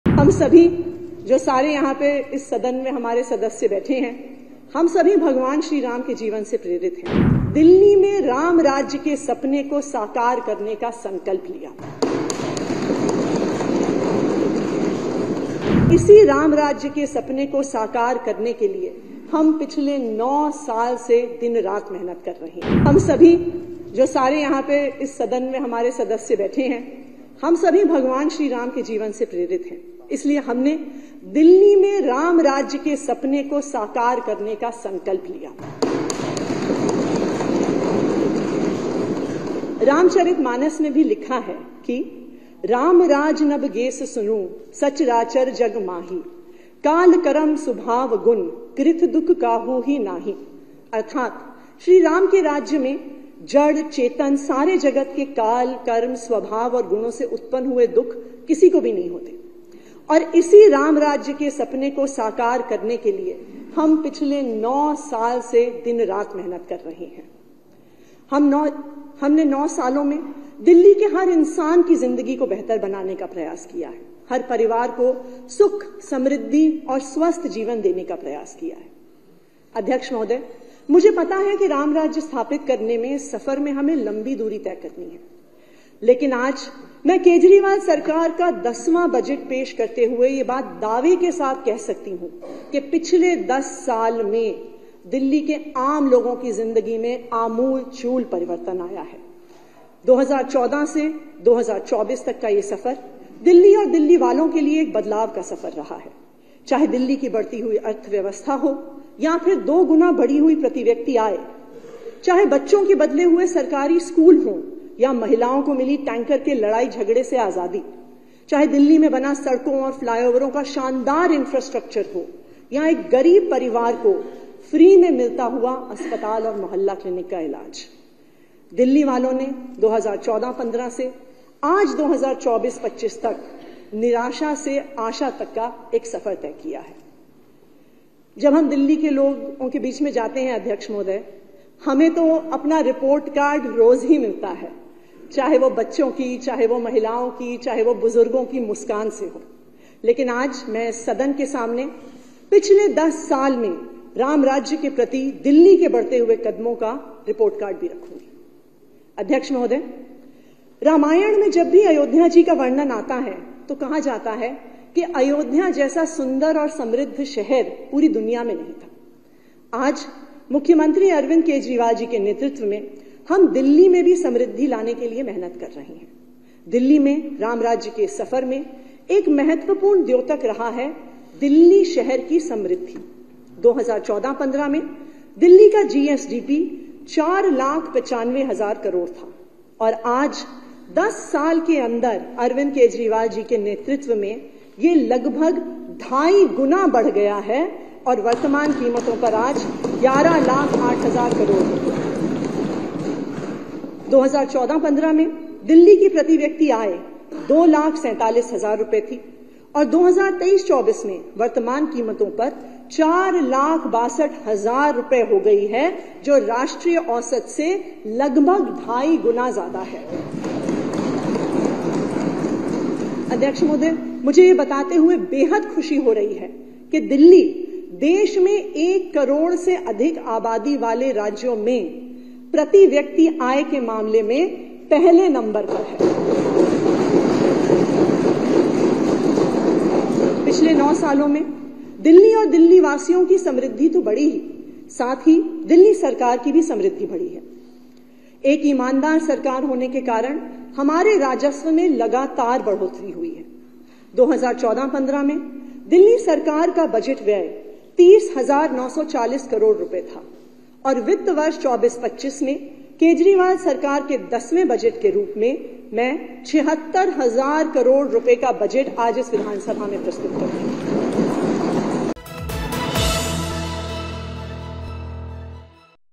Delhi Budget पेश करते हुए Atishi Marlena ने की कई घोषणाएं, राम राज्य का भी आ गया जिक्र
आम आदमी पार्टी (AAP) की सरकार आज दिल्ली का बजट पेश कर रही हैं। वित्त मंत्री आतिशी दिल्ली विधानसभा में अपना पहला और 'आप' सरकार का 10वां बजट पेश कर रही हैं। आतिशी ने अपना बजट भाषण में केजरीवाल सरकार की अबतक की उपलब्धियों के बारे में बताया। चुनावी साल में आ रहा वित्तीय वर्ष 2024-25 ‘राम राज्य’ की थीम पर आधारित है।